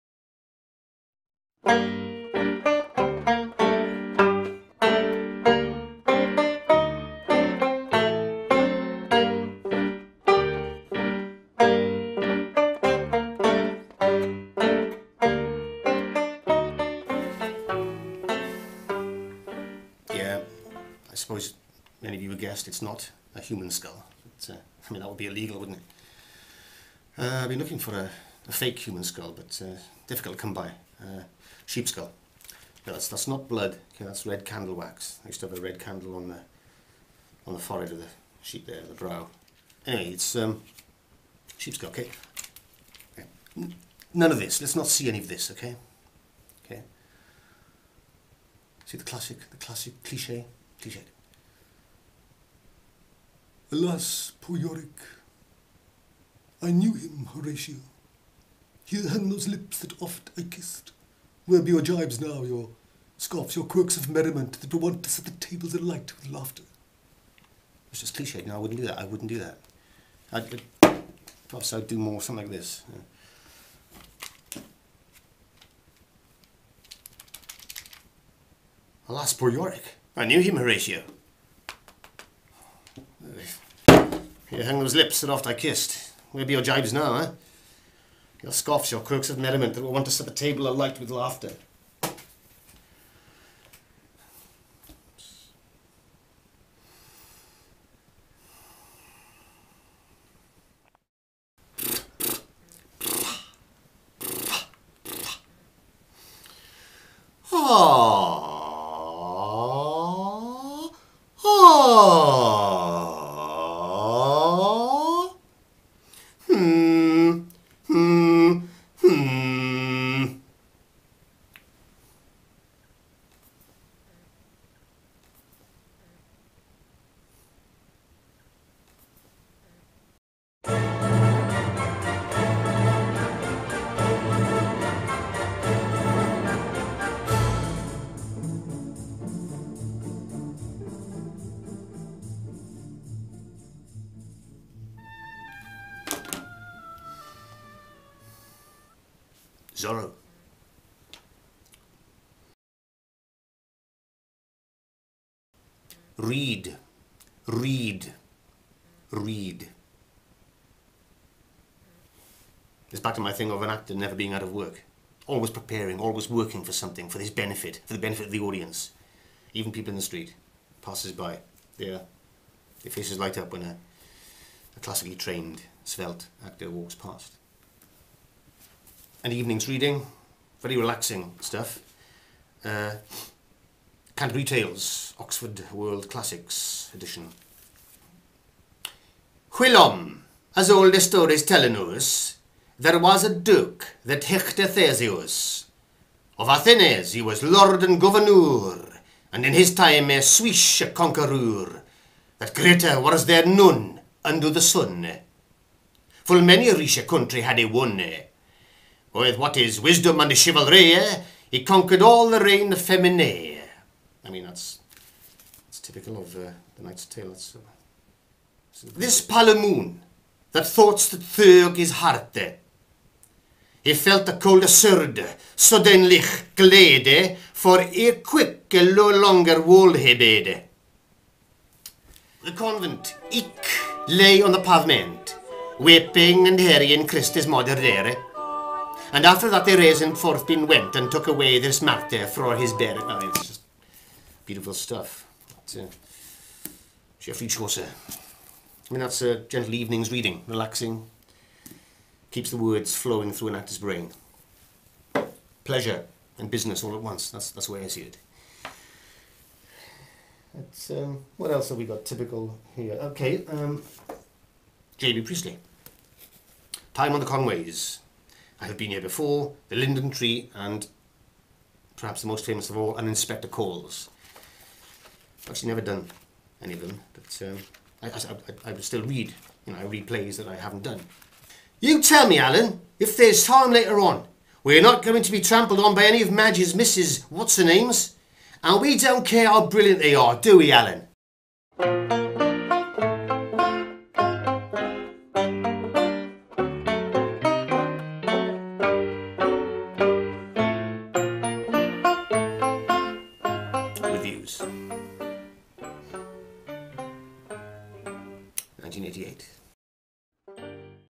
Episode 1: The Dane, Masks & Reading
First broadcast by classically trained, if unheralded, Welsh actor David Garland Jones.